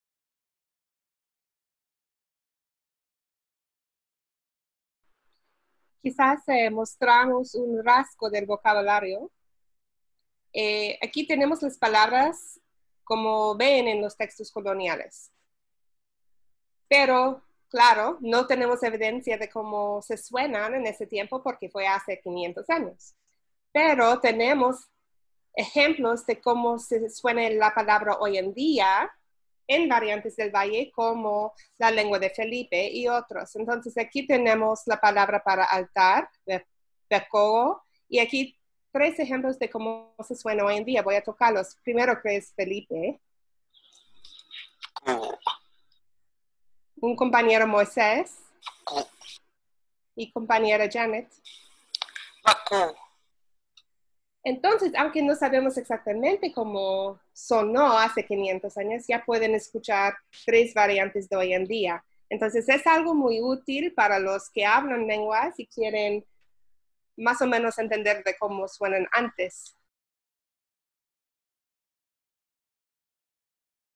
La página de vocabulario en Ticha
Dra. Brook Danielle Lillehaugen describe la página de vocabulario en Ticha.

Puedes aprender más acerca del sitio de Ticha en el primer capítulo de Caseidyneën Saën - Aprendemos Juntos

Este video es parte de una entrevista entre Dr. Clayton McCarl y Dr(a)s. Brook Danielle Lillehaugen, Xóchitl M. Flores-Marcial y Felipe H. Lopez.